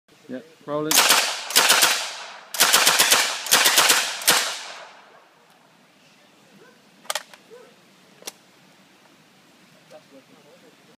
Test firing Bren Mk1
Here I am test firing a Mk1 Bren Gun. Using a magazine of 303, PPU blanks. We have modified the magazine to feed short blanks. It seems to work OK?
This video was taken by one of our armourers on his phone, so the quality is not the best.